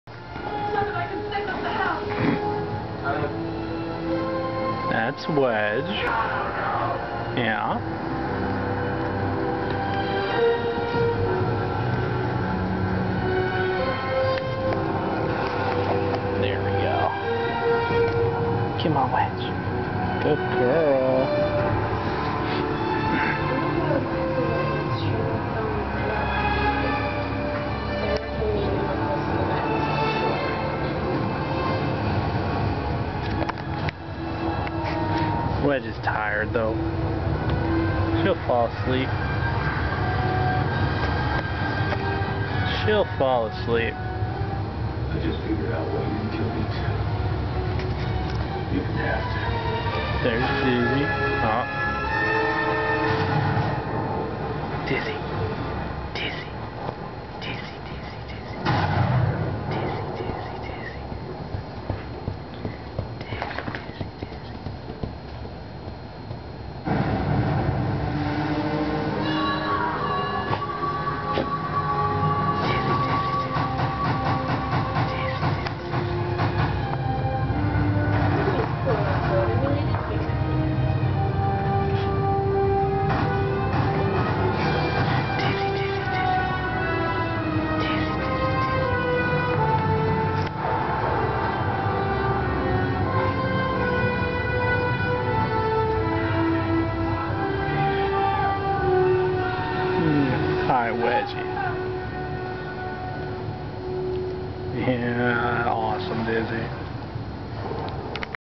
My cat, Wedge.
Wedge almost died from one of those topical flea treatments. we had put it on our dogs and we guess Biggs and Wedge cuddled up with the dogs and got poisoned. This is a video of Wedge after, as you can see he's doing fine now. Thank you to the local SPCA, my deceitful ex-girlfriend, as well as Jen and Lorin for all their help and support!